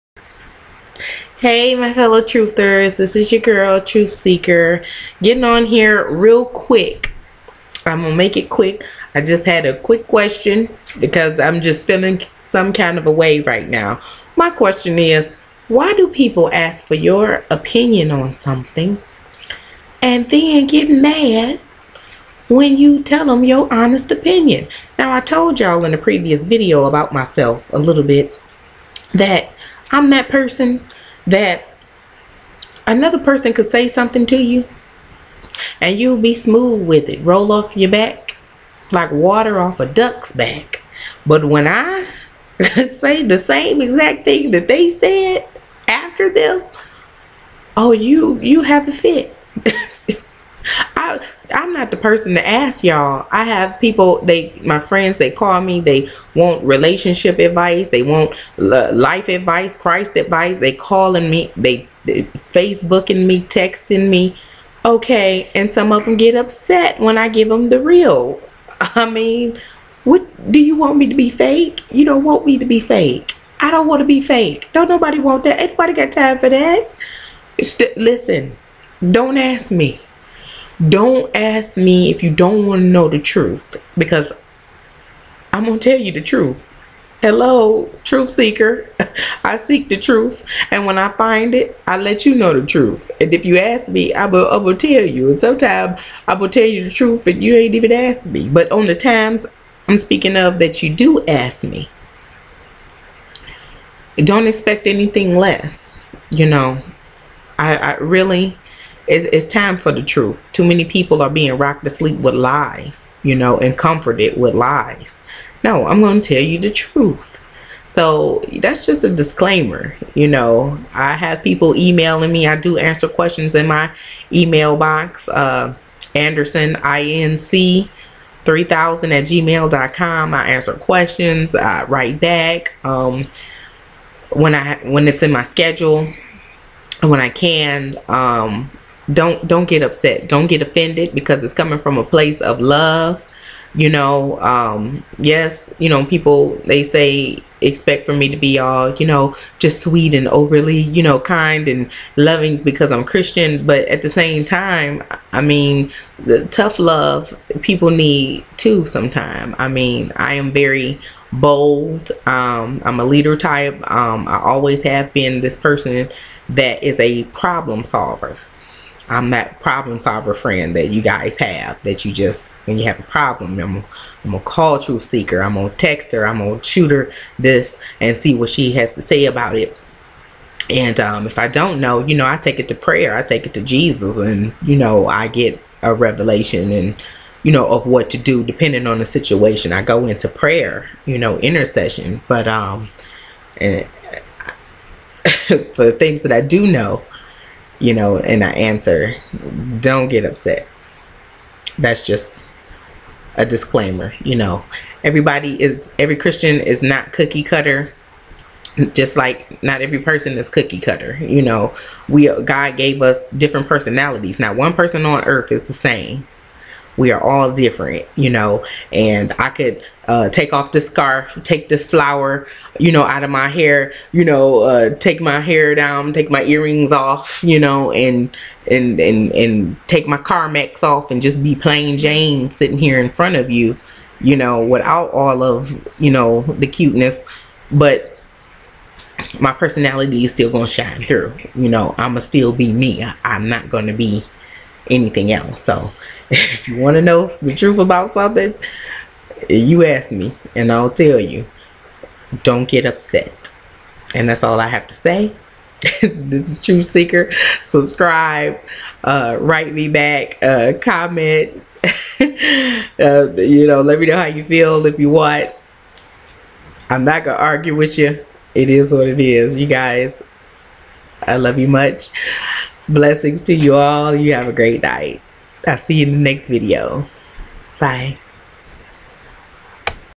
Why Do People Ask For The Truth but Get Upset When You Tell Them?
I just want to know why do People Ask You For The Truth But Then Will Get Mad At You For Telling them? Click here for more info on how to make $230 per person all day extra income. Just be prepared for the TRUTH honey! lol :)